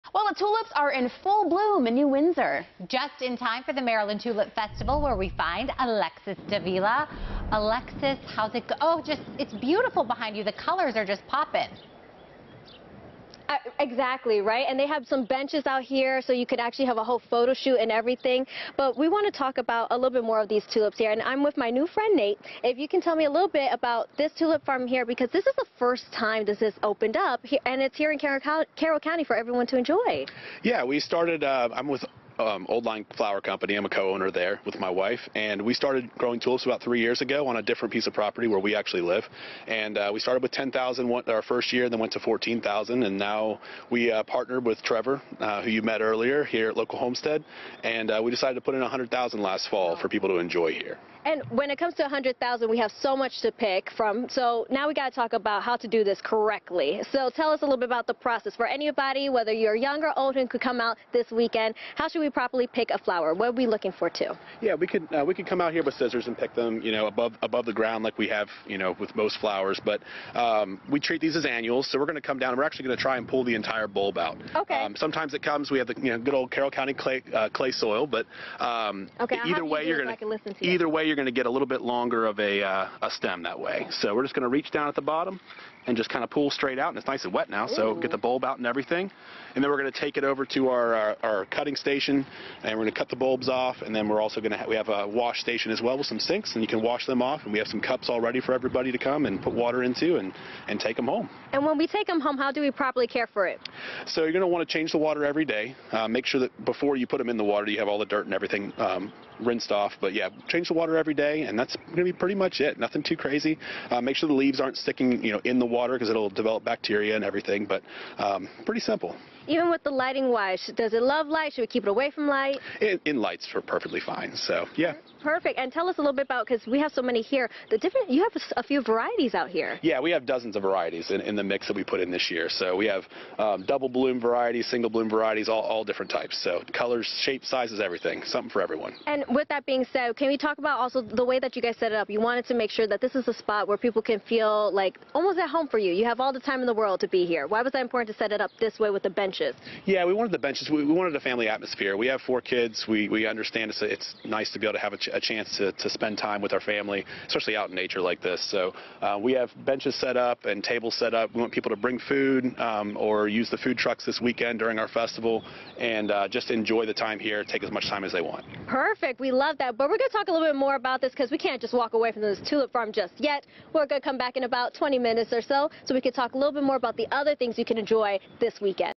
Over 100,000 tulips are on display at the inaugural Maryland Tulip Festival | Where's Alexus?
Tickets are $16 dollars in advance and includes 6 tulip stems and entry to the festival and kids are $8 with 3 tulip stems and entry to the festival.